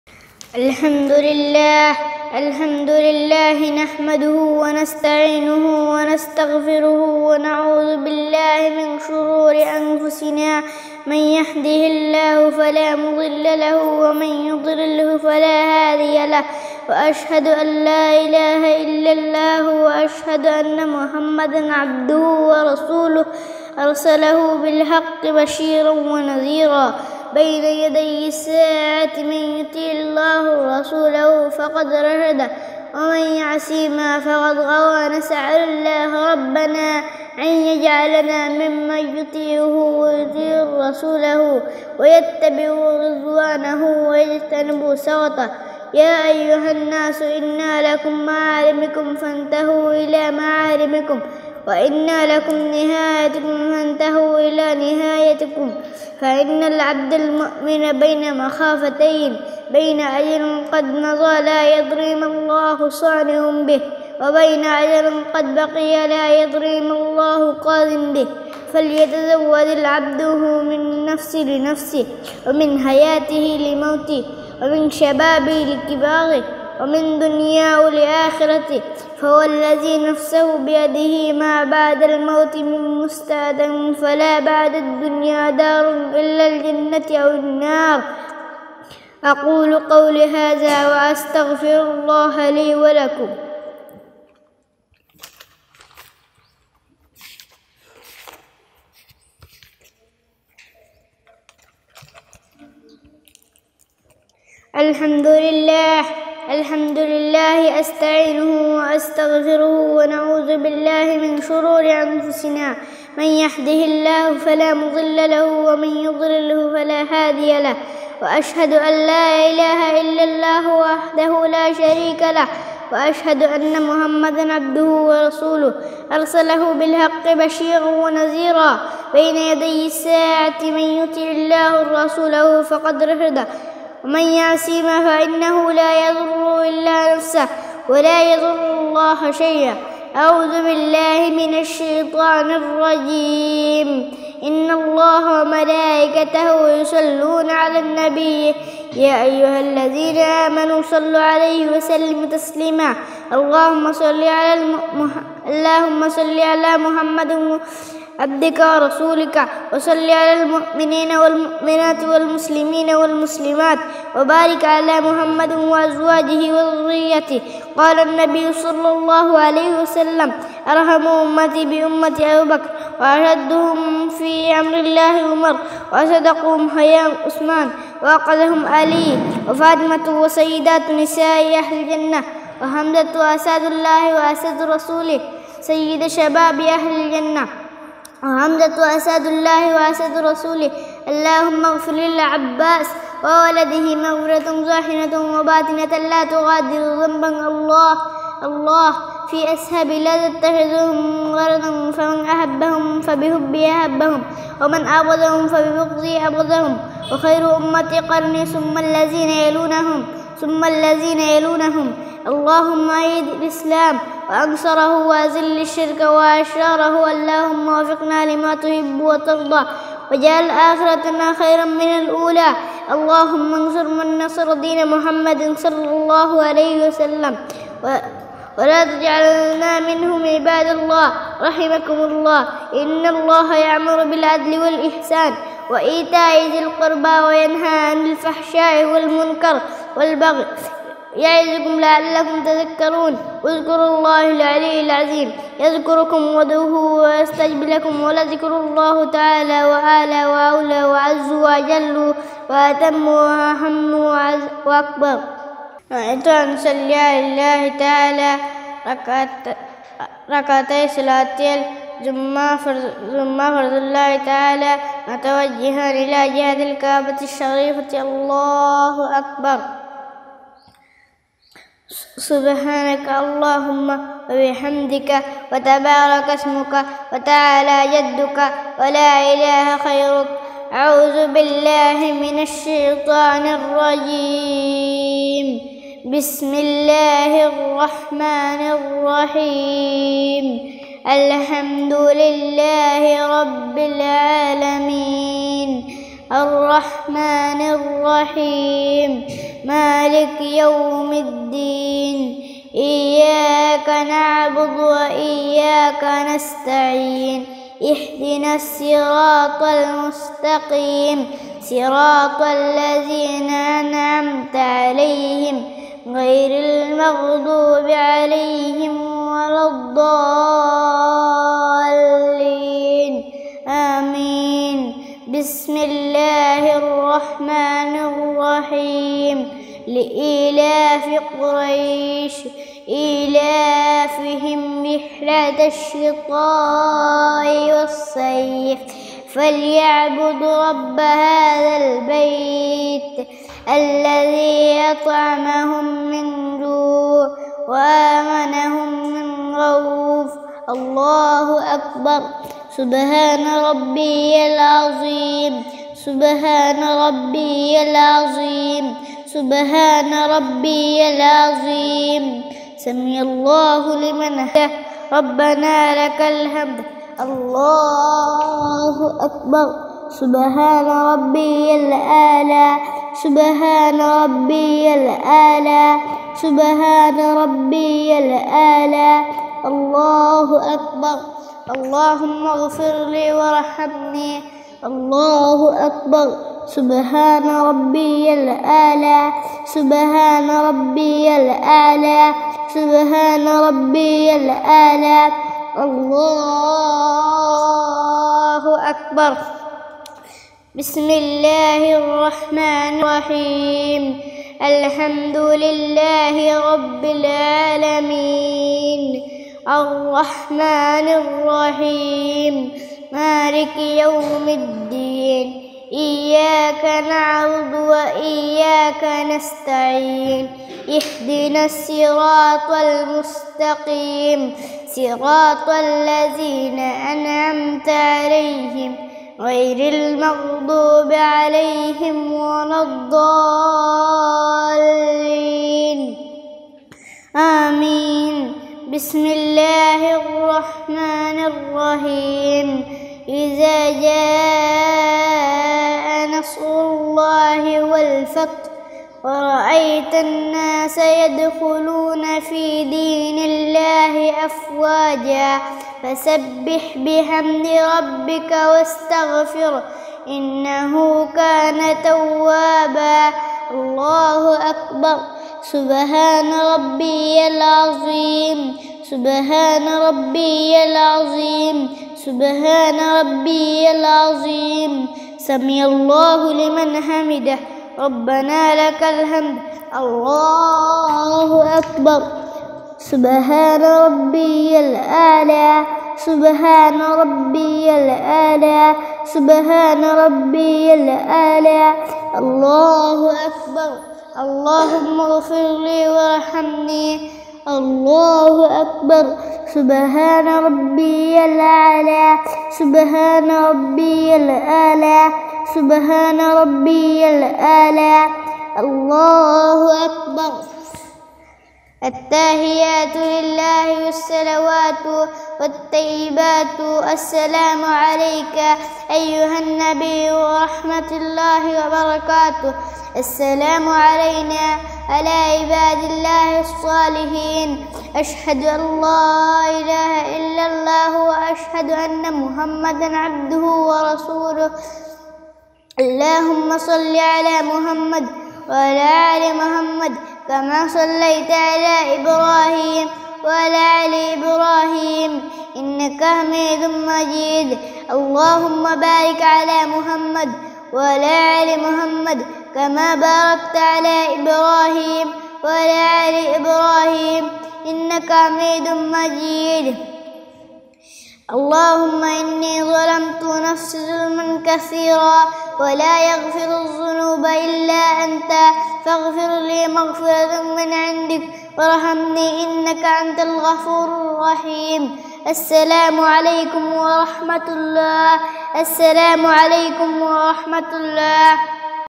৭ সাত বছরের বাচ্চা চমৎকার ভাবে জুম্মার নামাজ পড়ে দেখাচ্ছে ❤️ /কচারিপারা হুসাইনীয়া জামে মসজিদ ডবকা
৭ সাত বছরের বাচ্চা চমৎকার ভাবে জুম্মার নামাজ পড়ে দেখাচ্ছে কচারিপারা হুসাইনীয়া জামে মসজিদ ডবকা